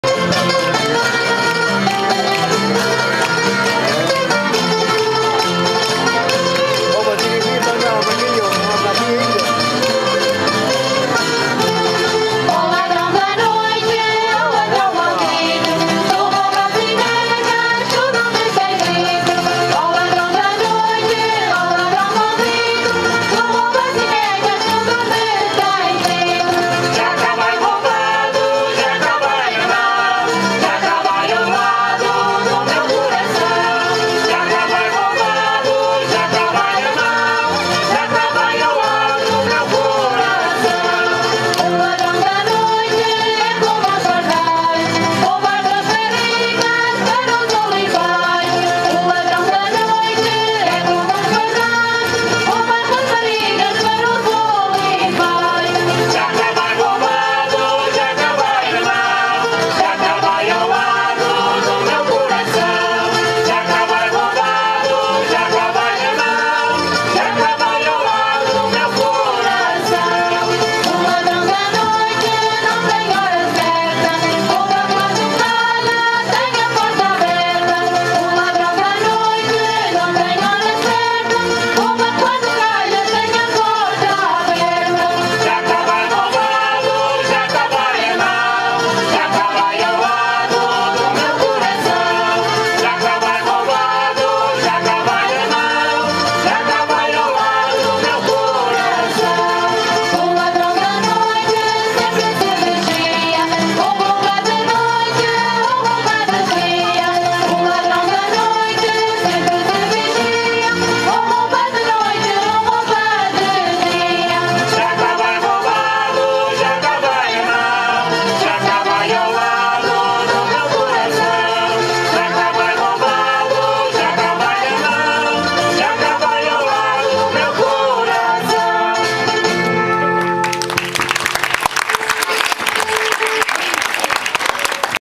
Grupo Folclórico da Casa do Povo de São João
Actuação do Grupo Folclórico da Casa do Povo de São João, no Jardim Municipal de São Roque do Pico.